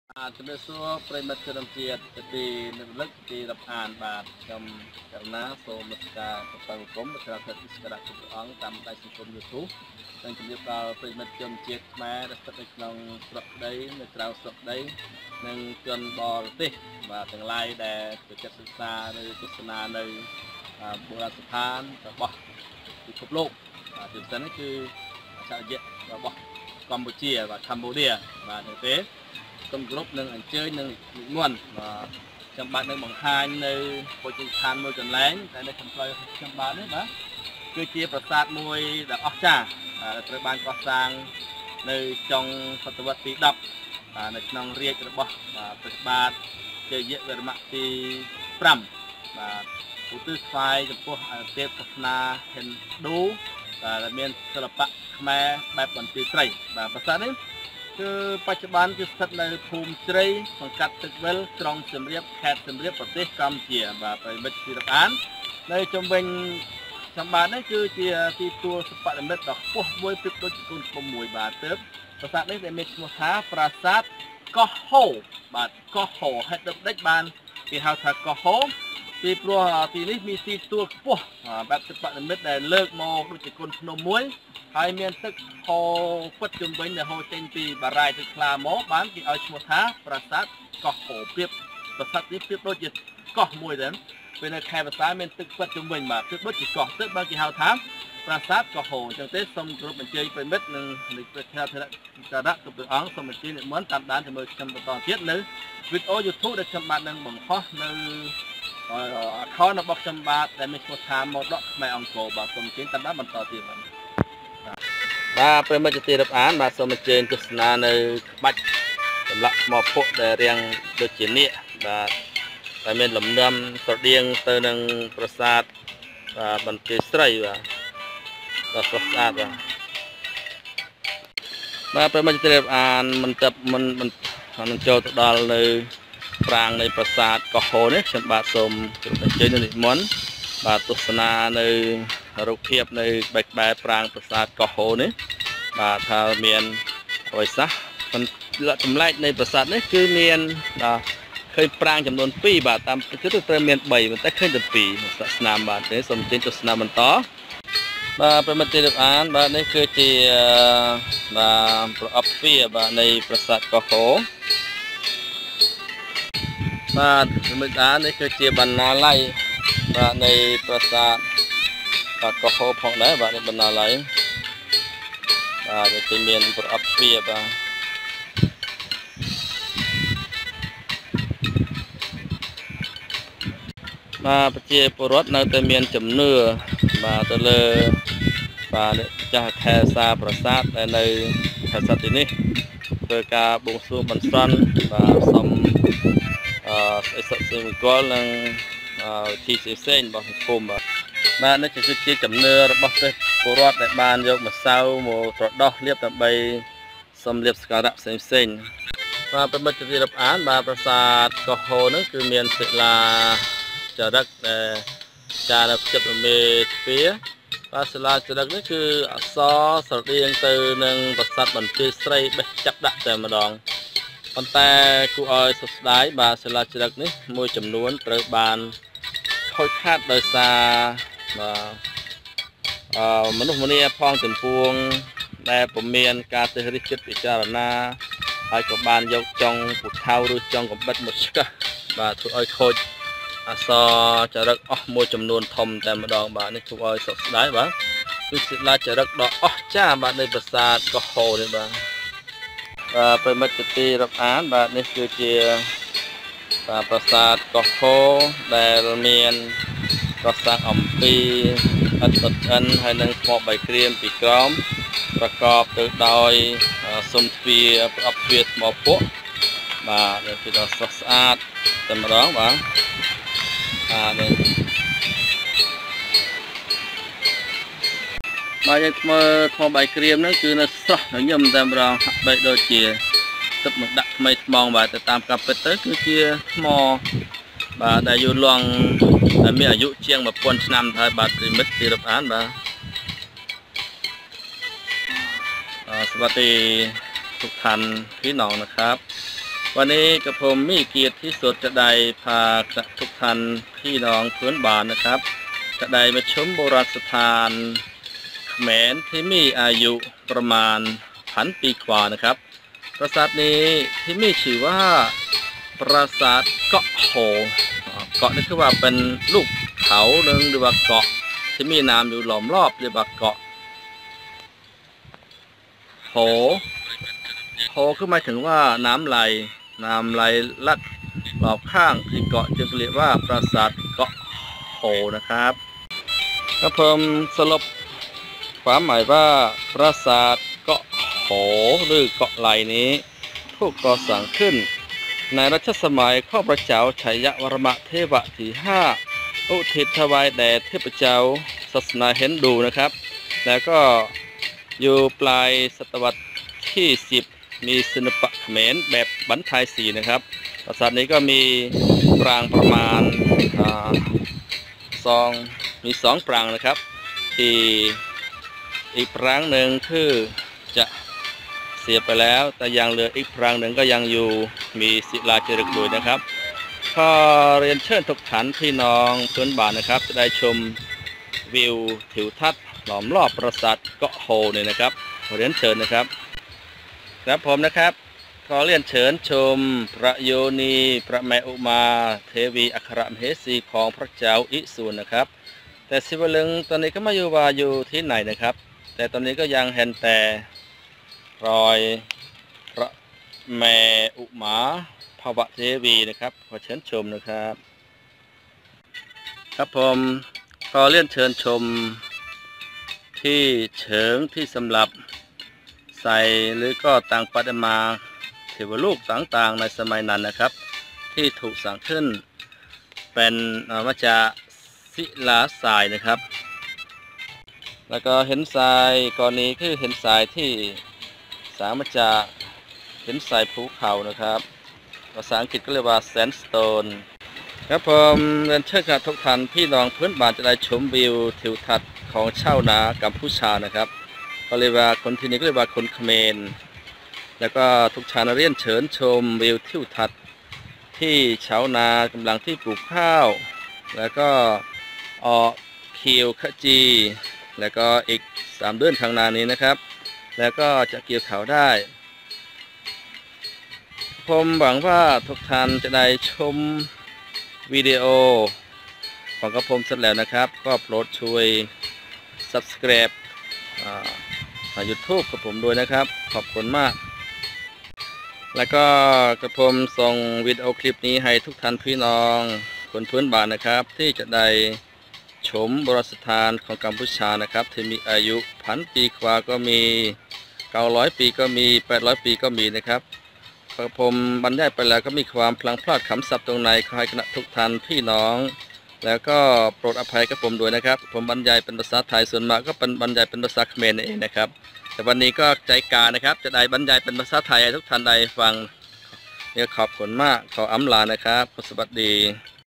ប្រាសាទកោះហូរ,,Koh Ho temple, MORODOK KHMER ANGKOR
ស្ថិតនៅភូមិជ្រៃសង្កាត់ទឹកវិលក្រុងសៀមរាបខេត្តសៀមរាបប្រទេសកម្ពុជា,MORODOK KHMER ANGKOR